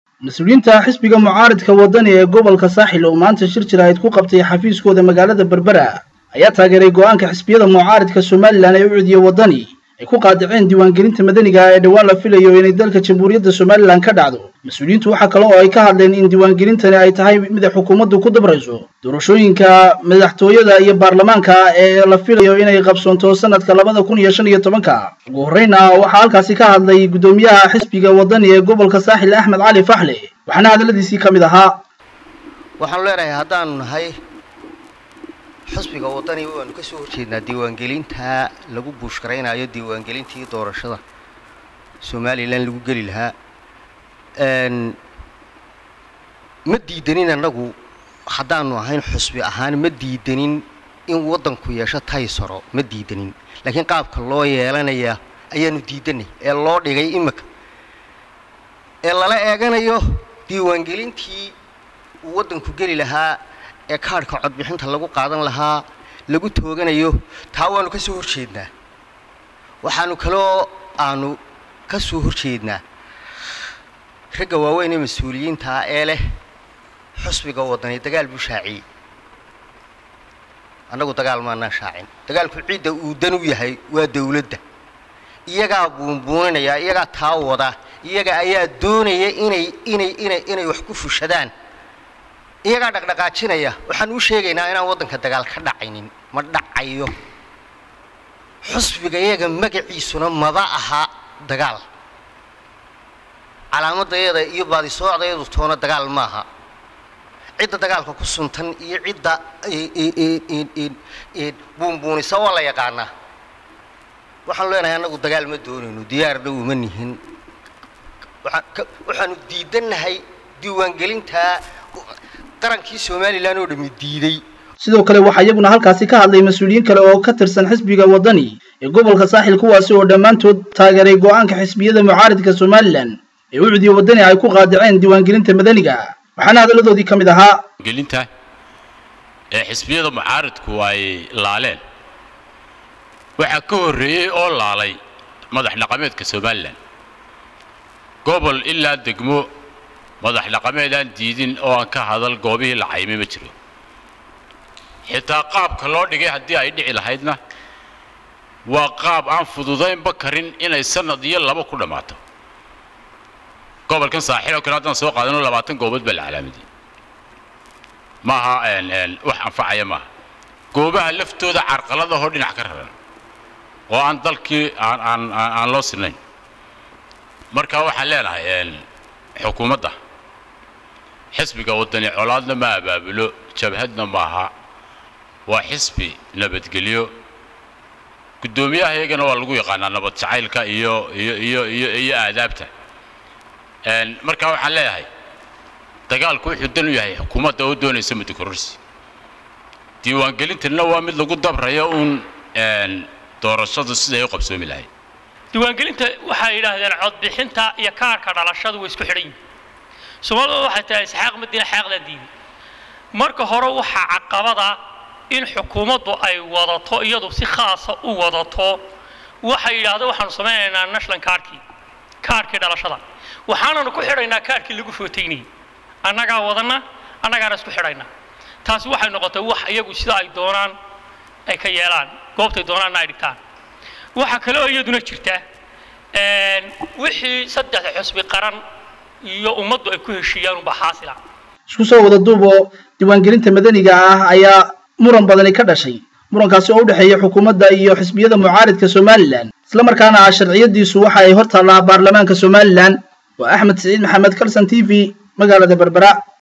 WARKA BERBERA EE XISBIGA WADANI
Xisbiga Wadani Ee Gobolka Saaxil Oo Manta  Shir Jaraa'id ku qabtay Xafiiskooda  Ayaa Tageeray Go'aankii Xisbiyada Mucaaridku Ku Qaadaceen Diwaan Galinta Madaniga